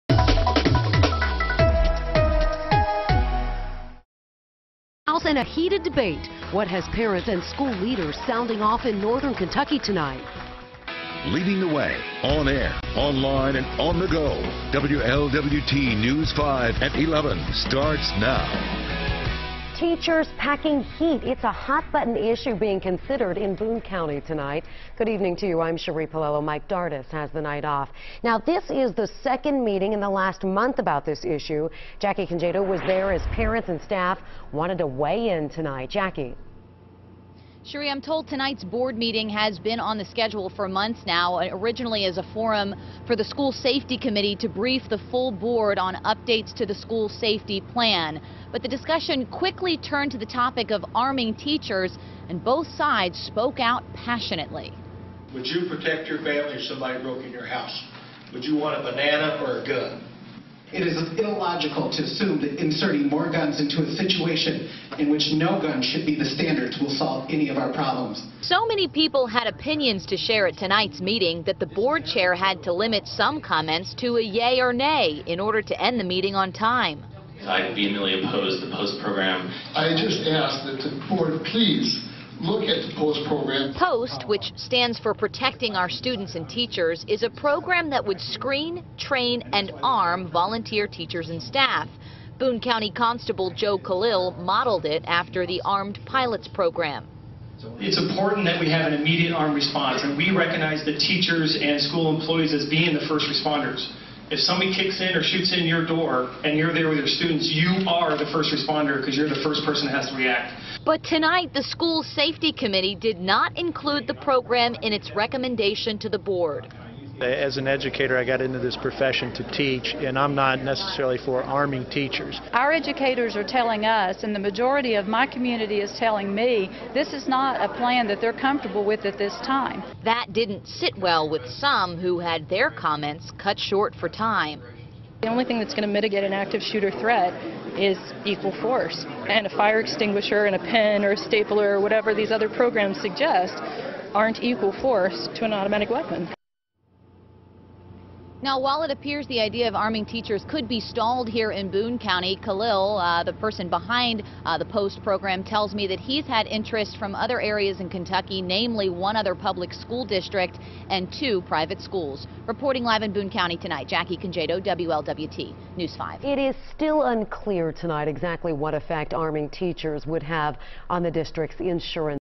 Plan to arm Boone Co. teachers all but stalled
The Boone County school district resumed discussions about arming teachers Thursday night.